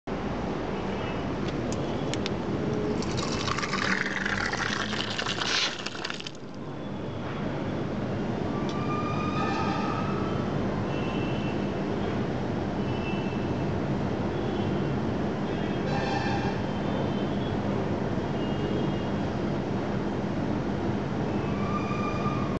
Kaltech Energy Solar Water Heater Jan 4th Shows 83.5 Degree Celsius.
Temperature Testing Video Of Pressurized Model Of Kaltech Energy Solar Water Heater On Jan 4th Shows 83.5 Degree Celsius.